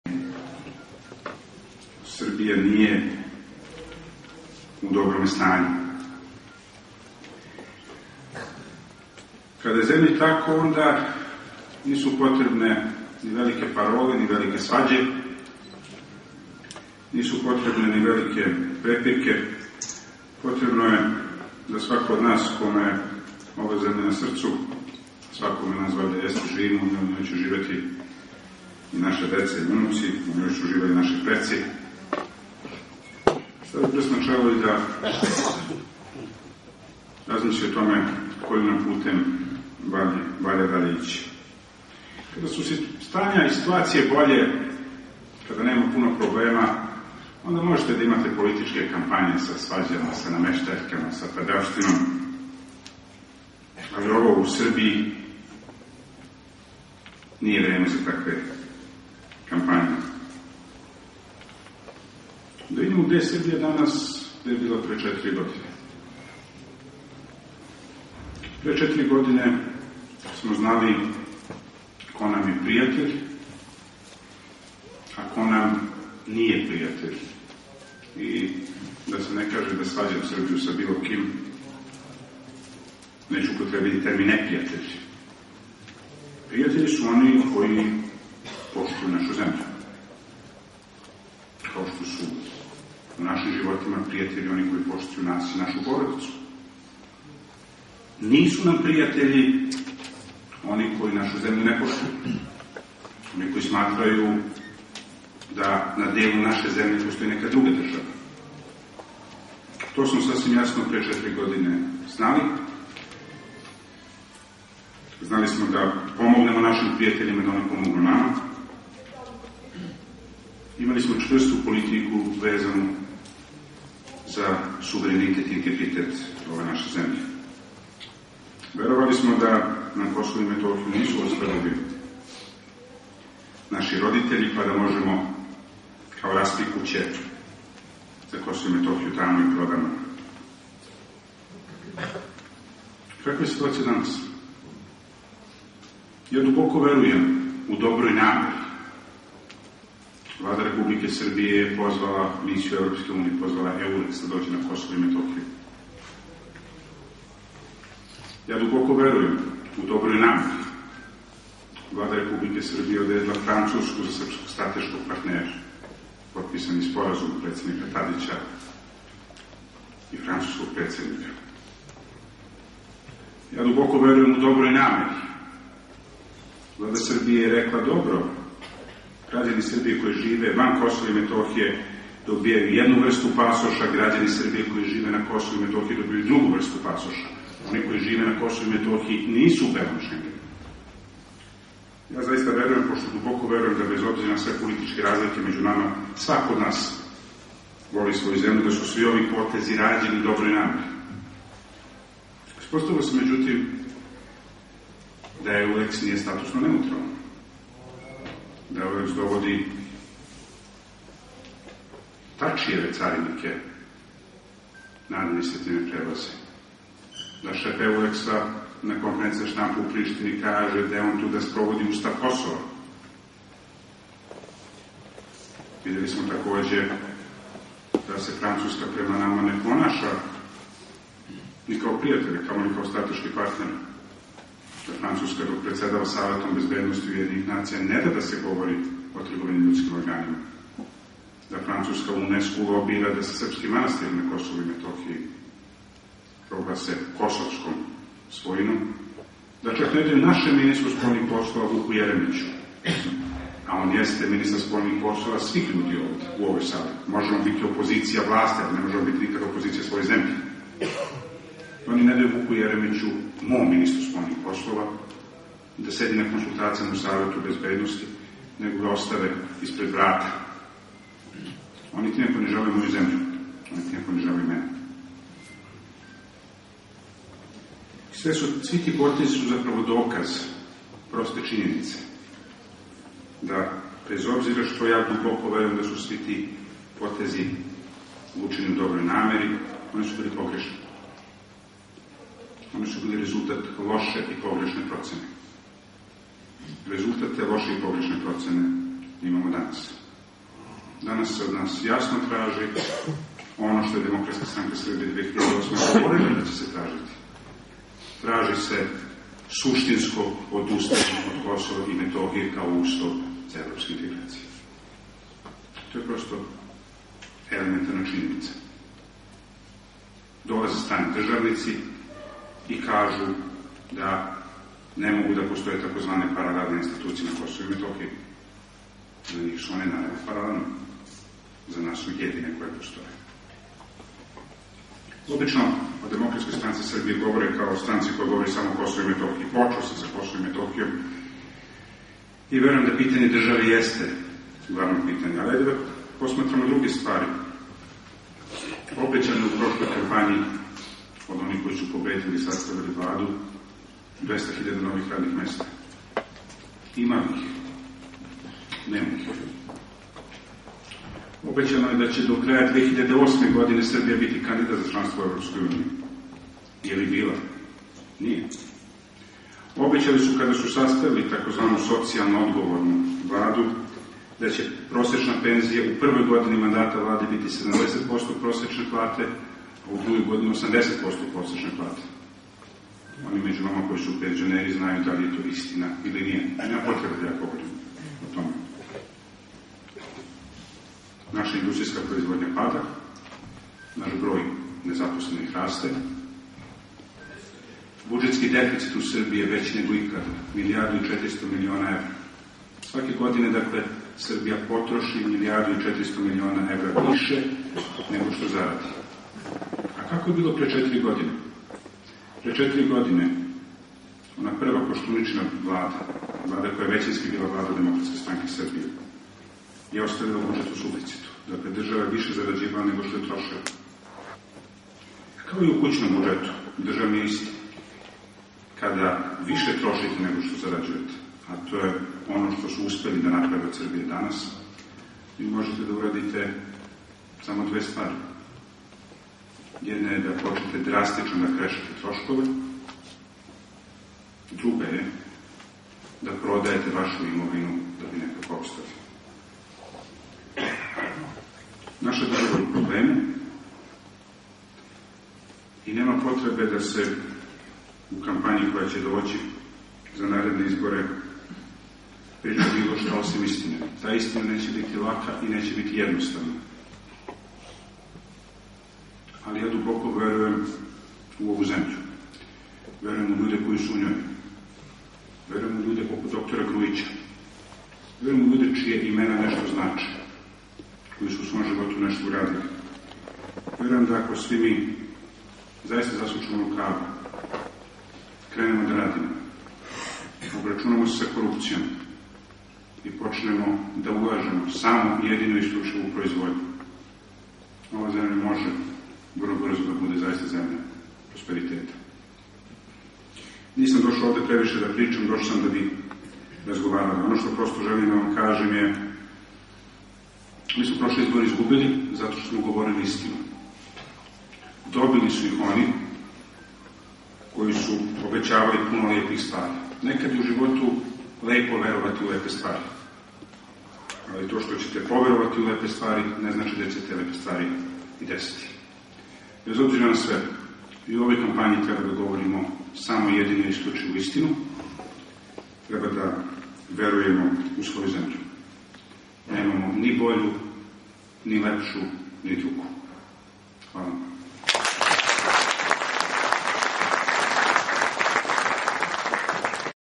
2011-10-18: Са трибине ДСС "Помозимо Србији" (2. део)
Са трибине ДСС "Помозимо Србији" која је одржана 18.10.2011. у Смедеревској Паланци. Гост вечери је био Александар Поповић, потпредседник ДСС.